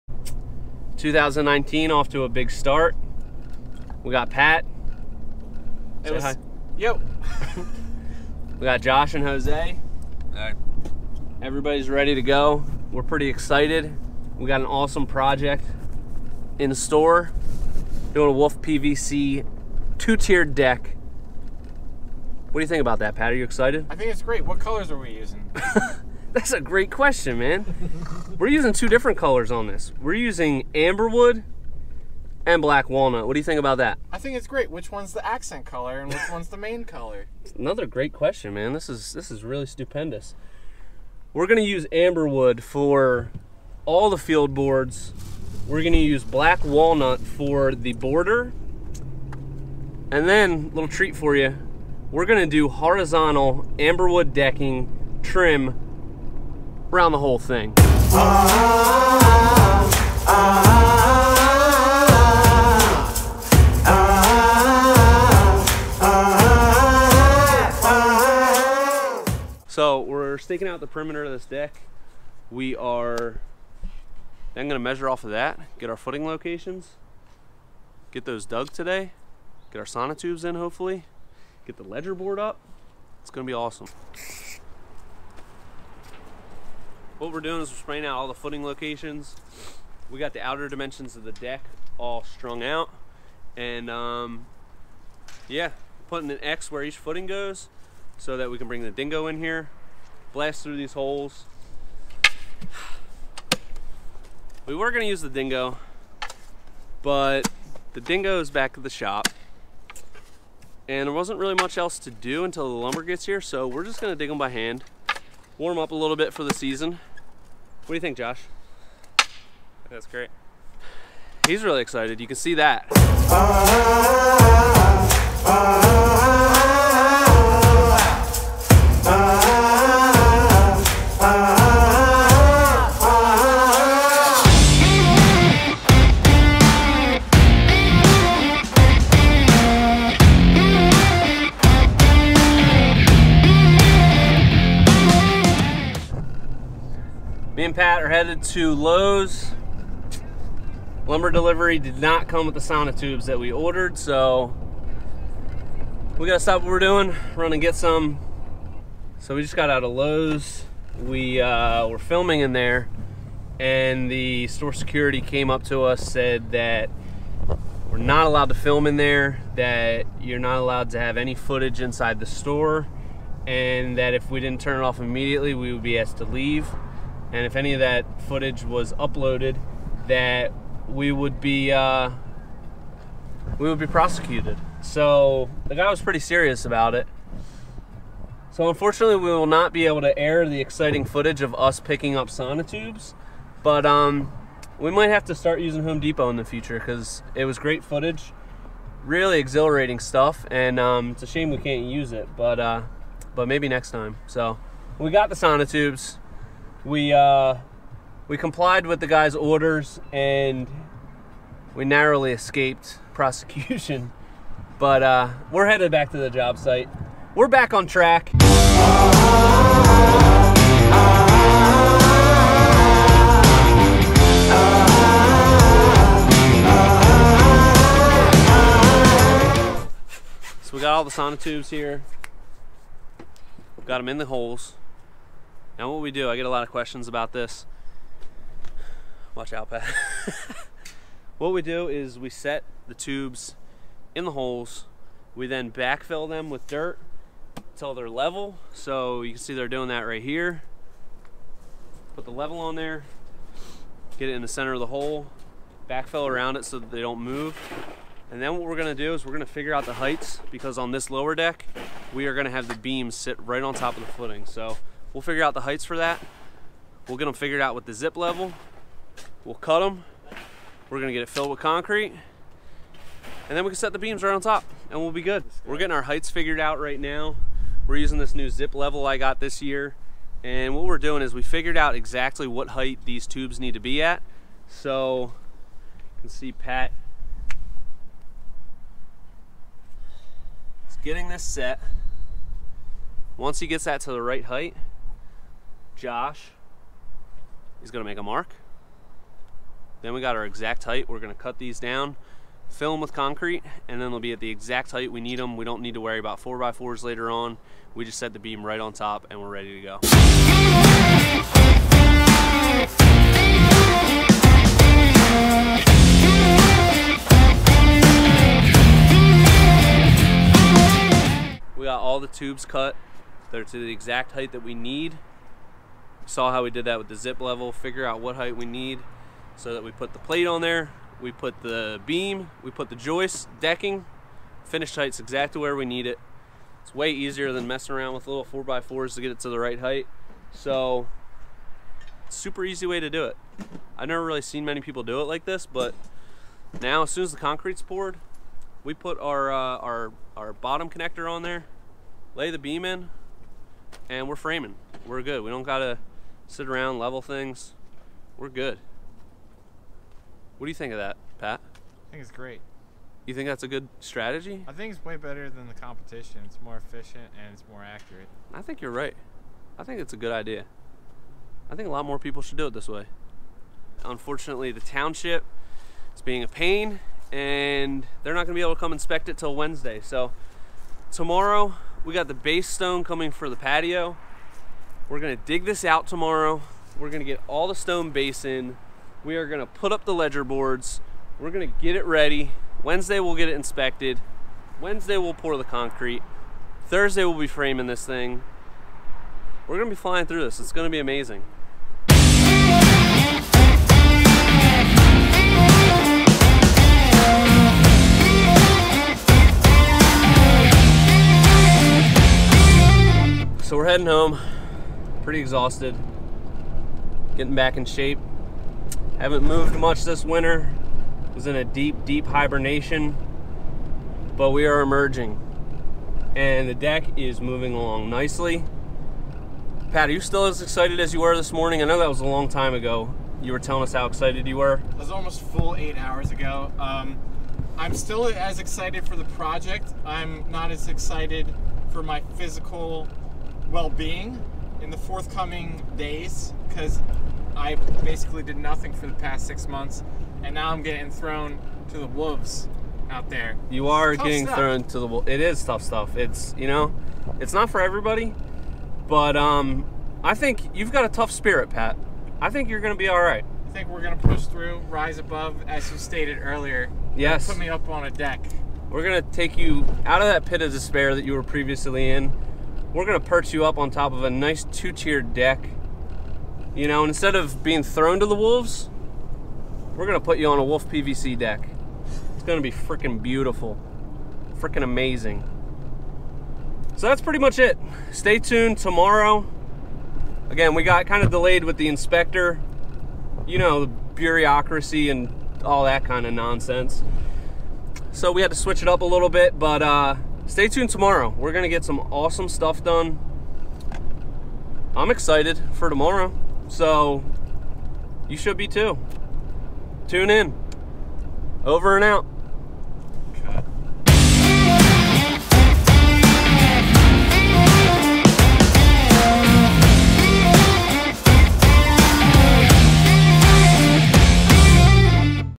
2019 First Day! Layout and Footings on a Wolf PVC Deck
First day of our 2019 season, we are laying out the deck and the footings on a beautiful Wolf PVC deck, check back in tomorrow for the next step!